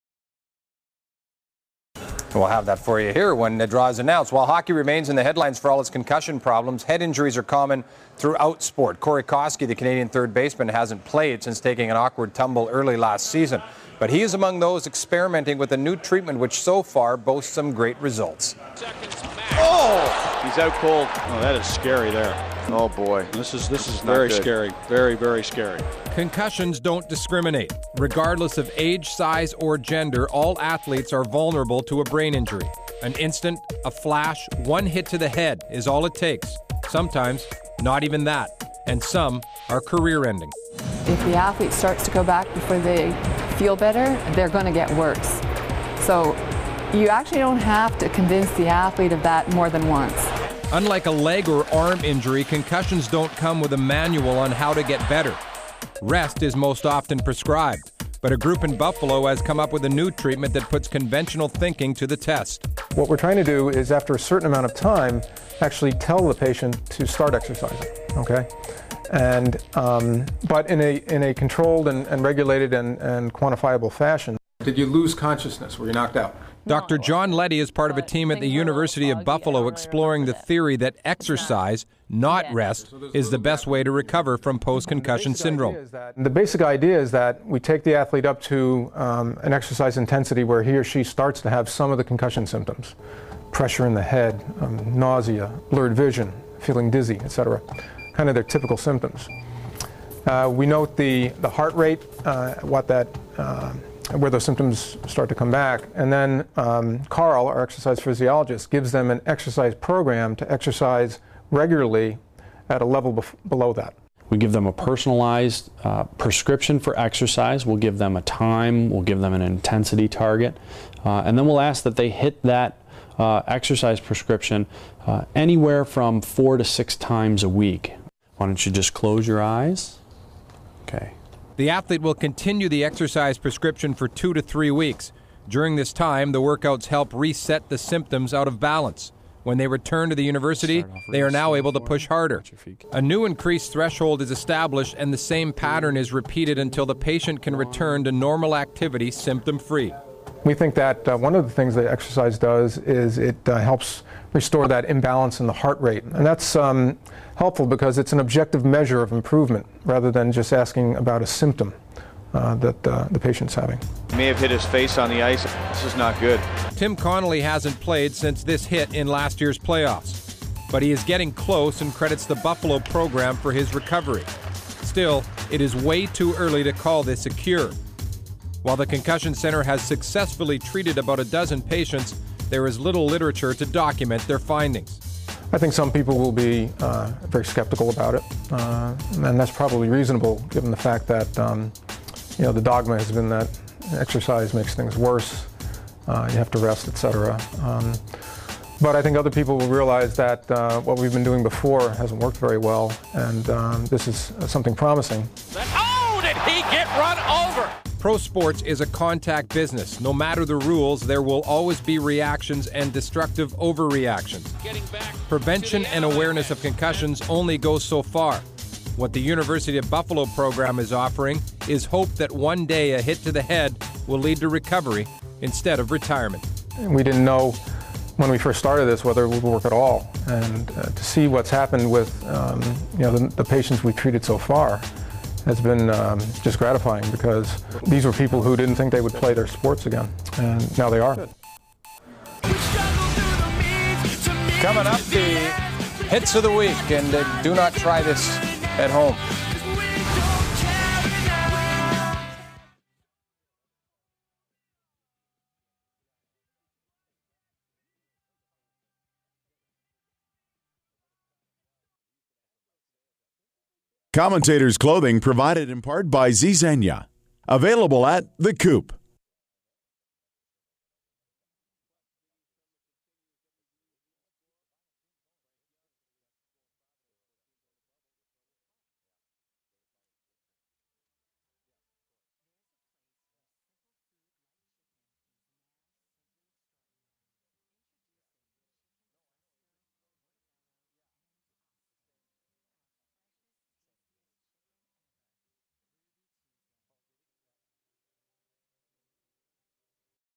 UB Public Health and Health Professions: Exercise and Concussions
See what researchers at the University at Buffalo's School of Public Health and Health Professions are doing with Concussion victims.  Exercise is the medicine!